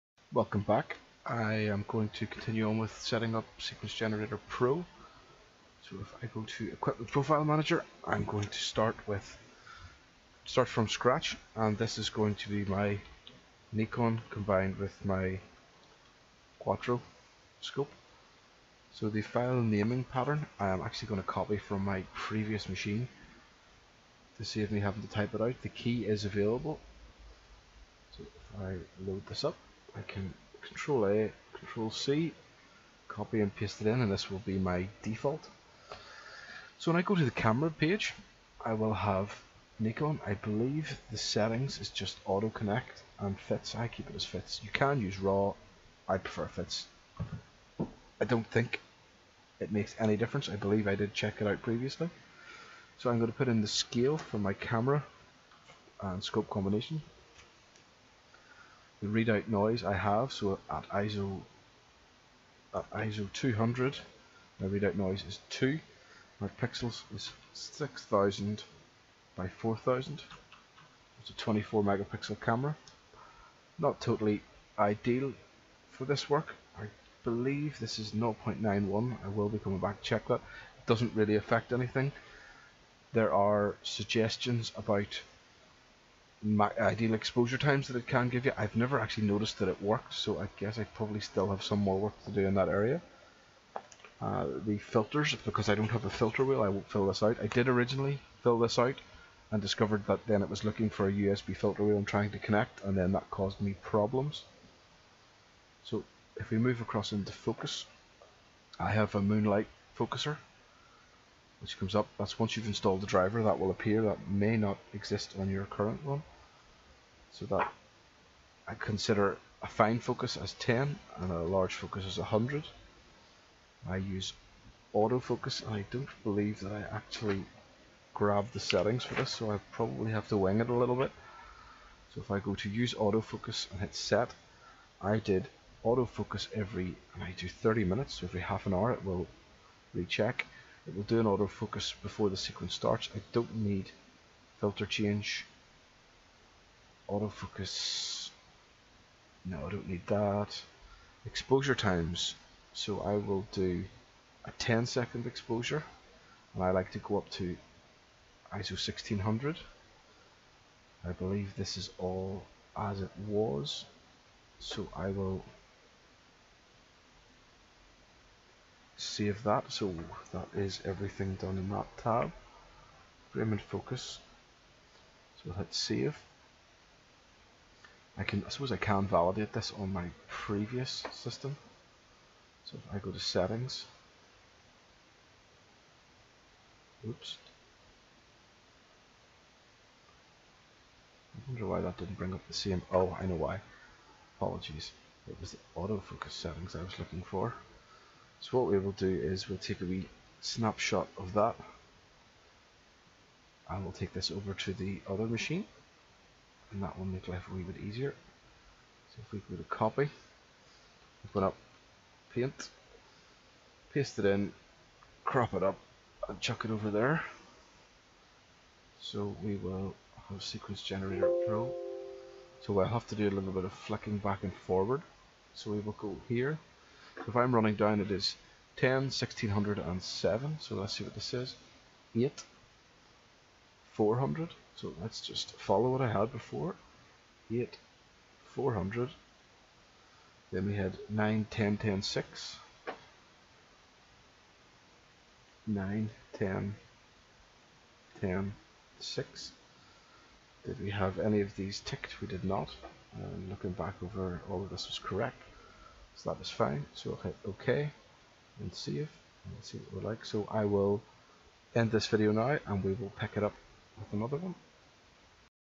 How to: SG Pro Configuration Pt 3 [Equipment Profile: Camera & Focuser]
A video showing configuration of the following areas of Sequence Generator Pro:

- Equipment Profile Manager
     + Camera
     + Focus